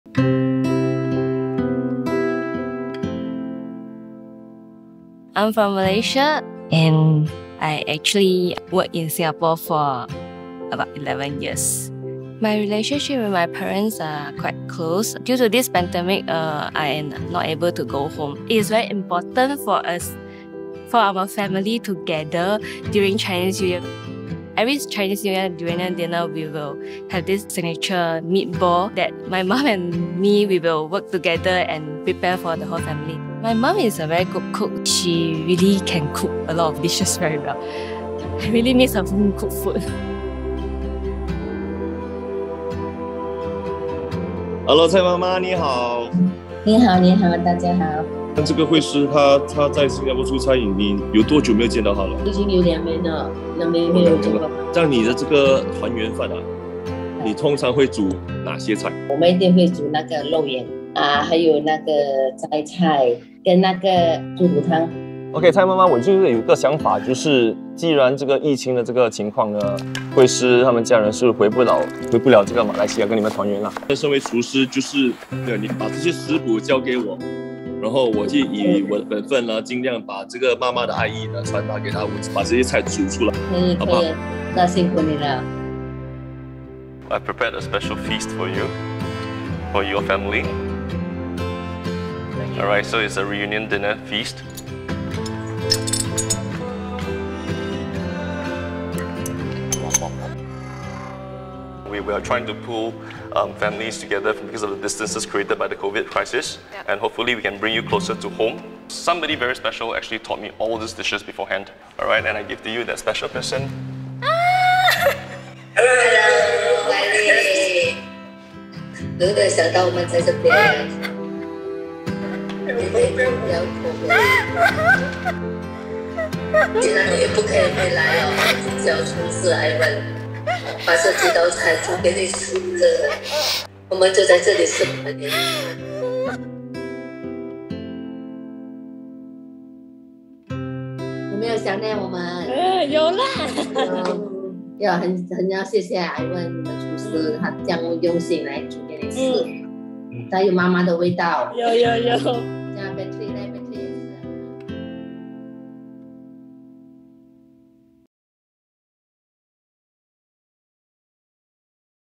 A Taste of Home: Savour Your Moments of Reconnection
This Lunar New Year, how will you reconnect with those who matter most? Nothing brings families
together like a sumptuous, home-cooked reunion dinner. It might be a tradition for most but for some, it’s a precious opportunity to reconnect with loved ones – especially when they are miles apart.